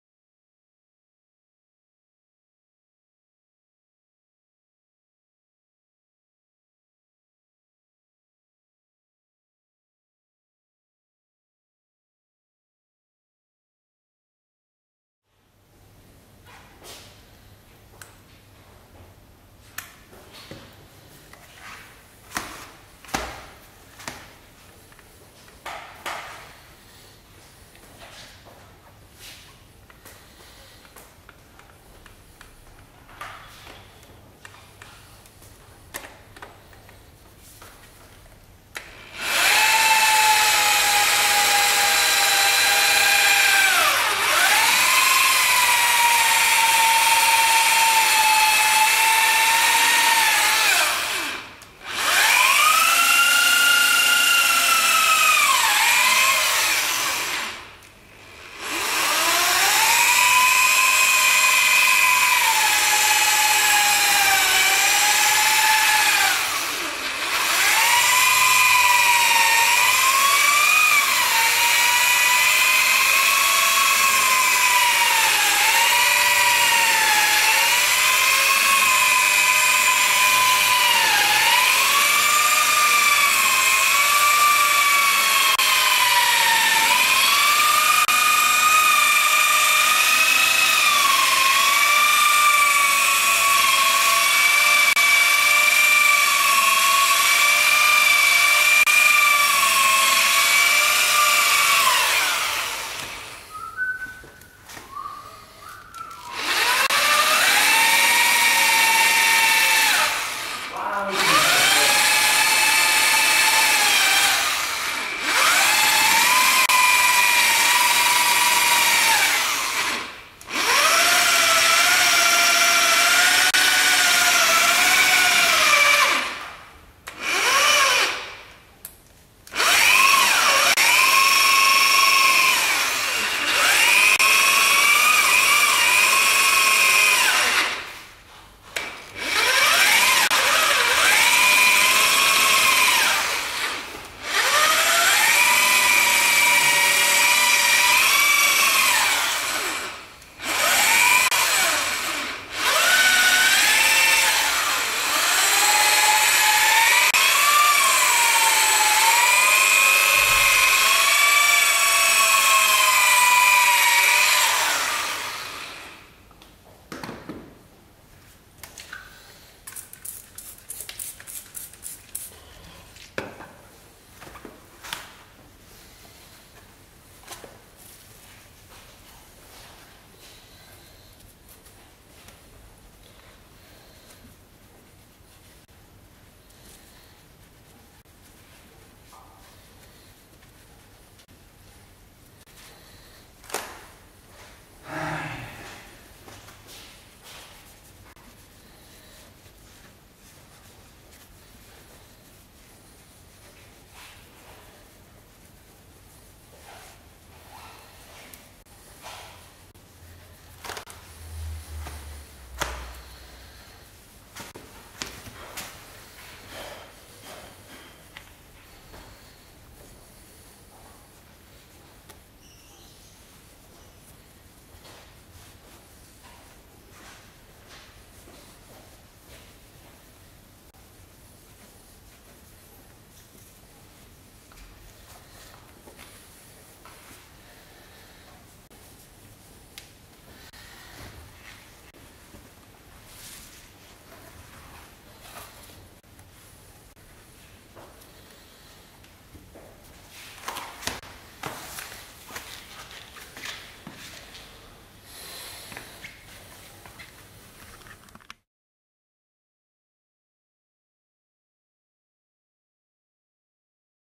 Pulido de llantas con Flexipads bola de pulir llantas
Uso de la bola de pulir llantas de Flexipads + Menzerna Power Finish para restaurar el brillo de la llanta.